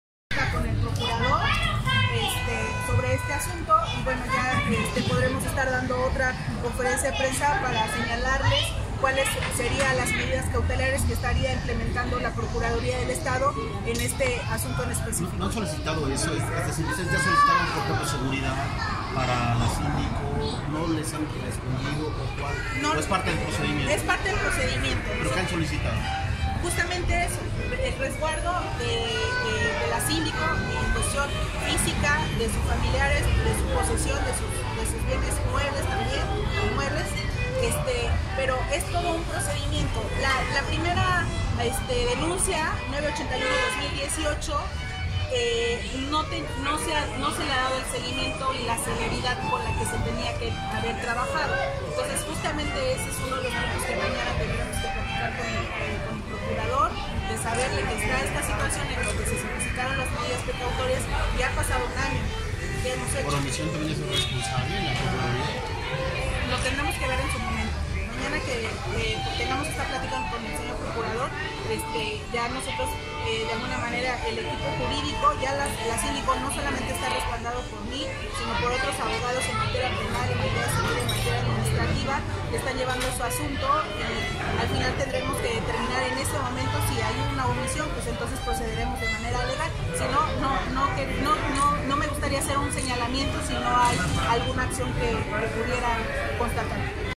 Pide Síndica de Huactzinco a PGJE medidas cautelares en su favor
Visita nuestro portal:
Luego de que sujetos desconocidos balearon su domicilio.